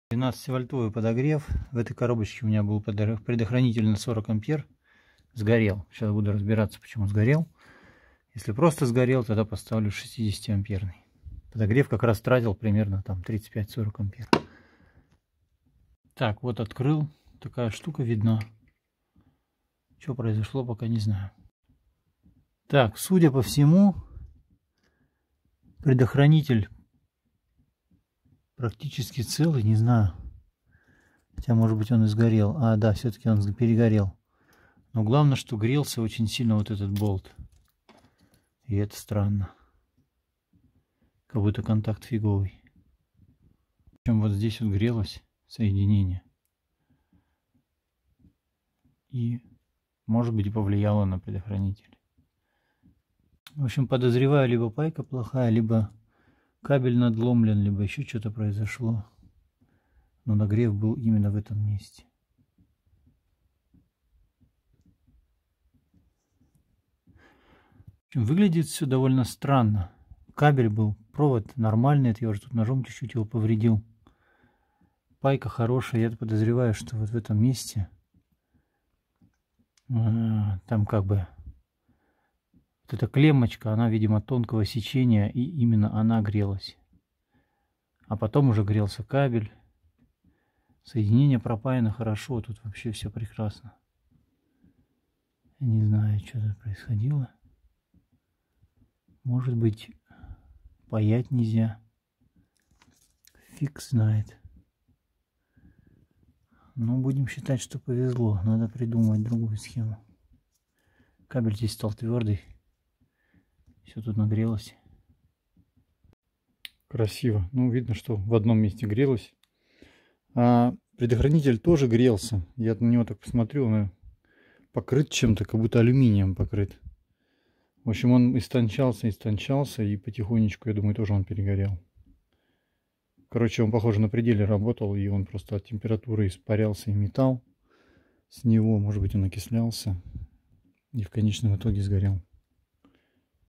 Сгорел предохранитель на 40А, но похоже ему помогли.
предохранитель заменил на японский